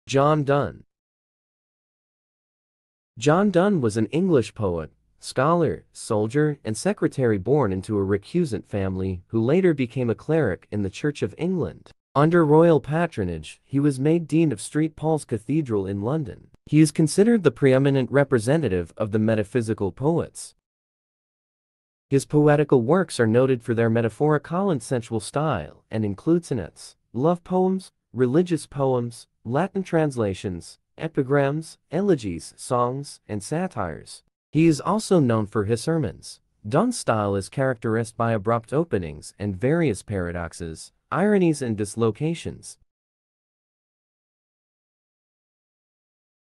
John Donne English poet|| English Literature || English Learning || English Department
John Donne was an English poet, scholar, soldier and secretary born into a recusant family, who later became a cleric in the Church of England. Under royal patronage, he was made Dean of St Paul's Cathedral in London. He is considered the preeminent representative of the metaphysical poets.